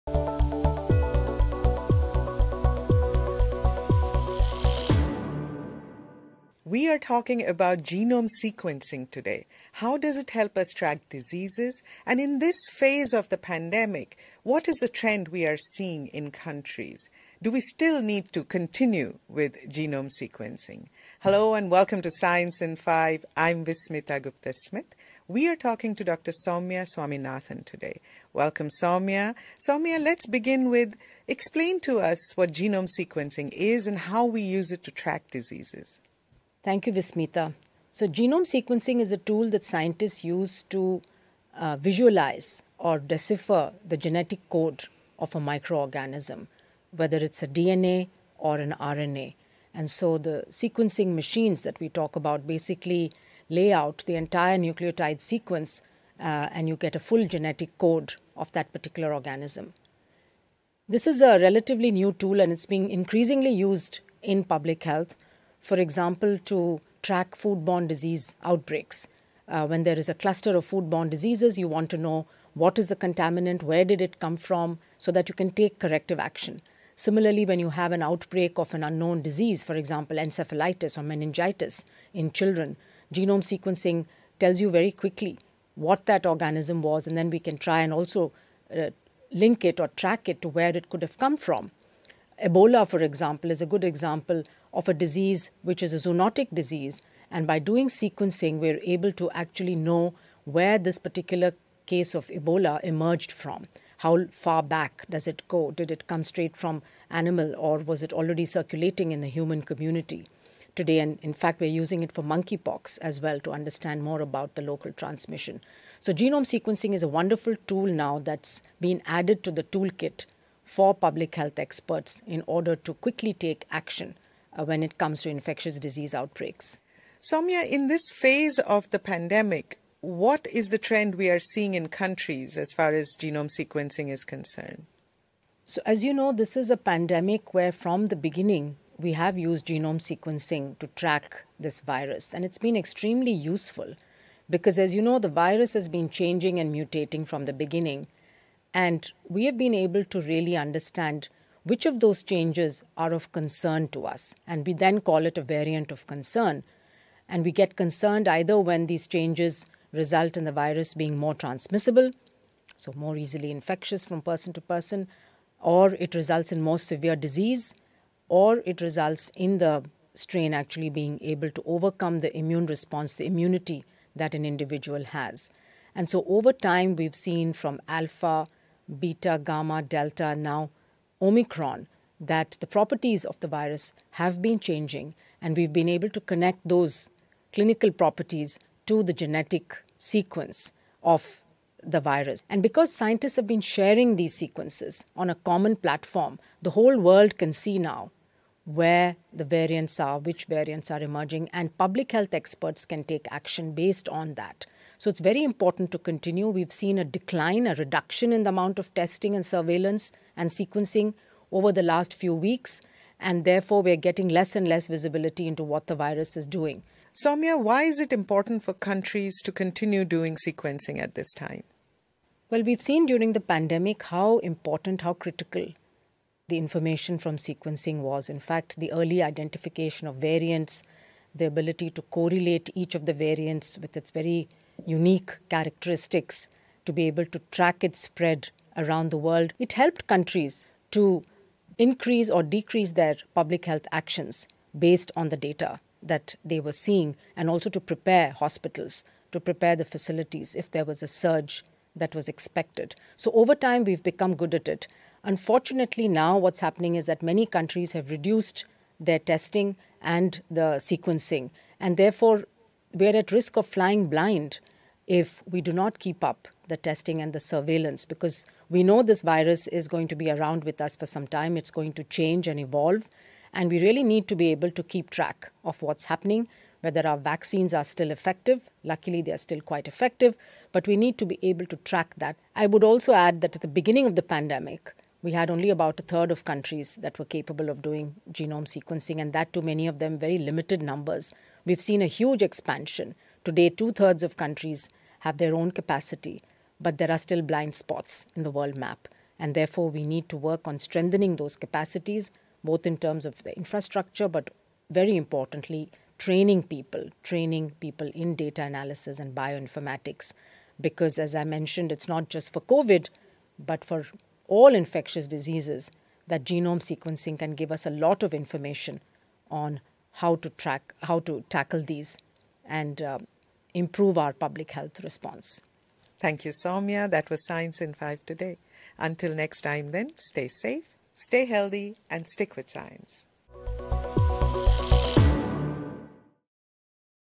What Is Genome Sequencing And How Does It Help Us Track Diseases?
What is genome sequencing and how does it help us track diseases? Why do countries need to continue genome sequencing in this phase of the pandemic? WHO's Chief Scientist Dr Soumya Swaminathan explains in Science in 5. [Credit: World Health Organisation]